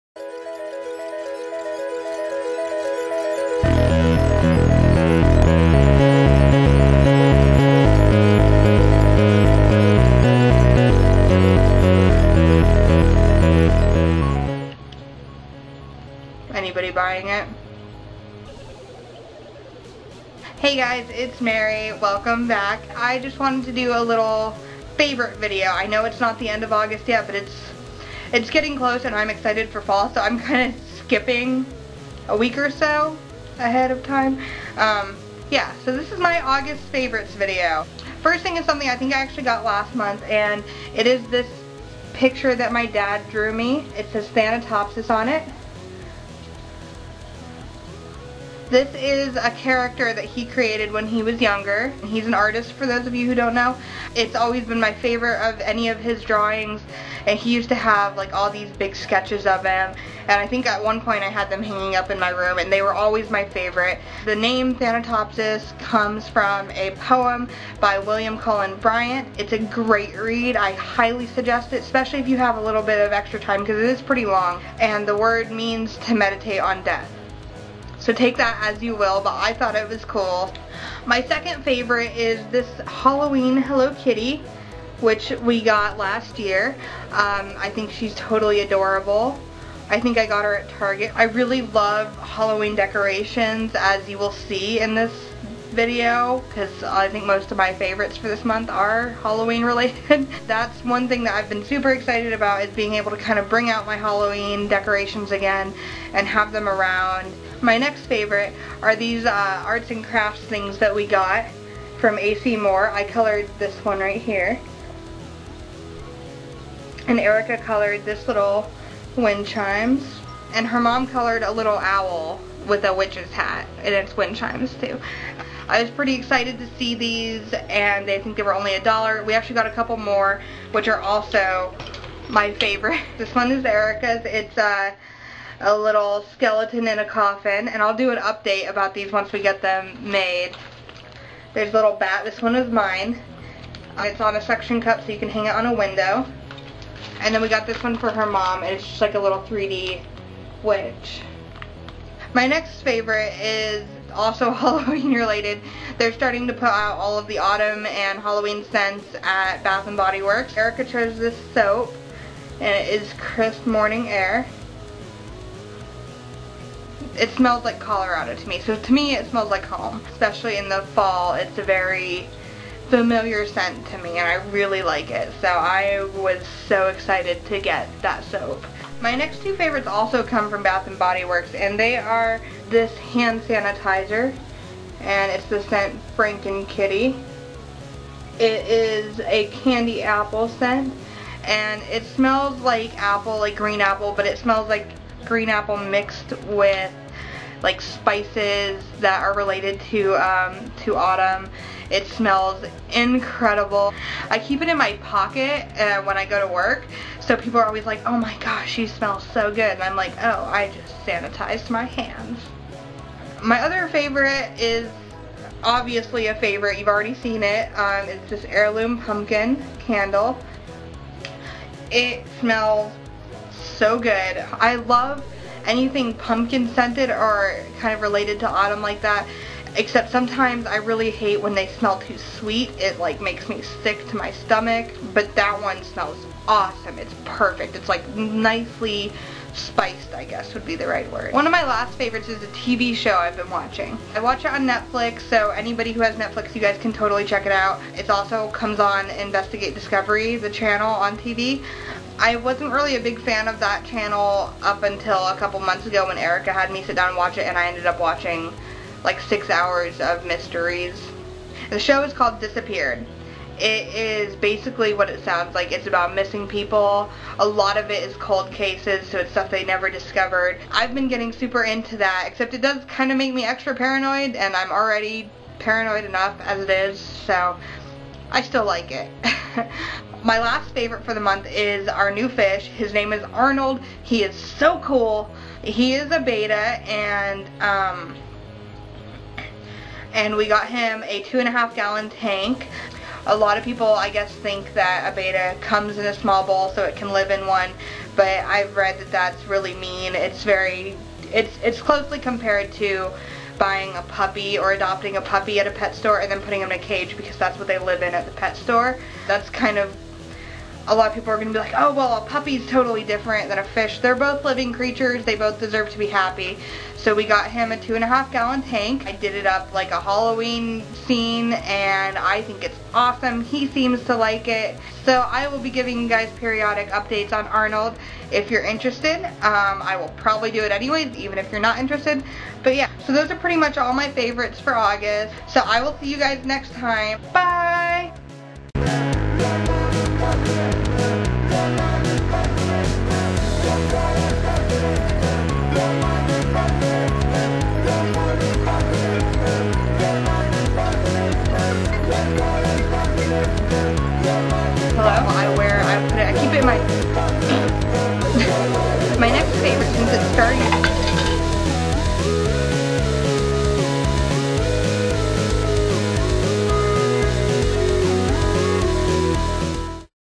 MARY'S AUGUST 2014 FAVORITES!!!!
Literally what the title says. Mary obsesses over Halloween decorations even though it's not even October yet. Deal with it.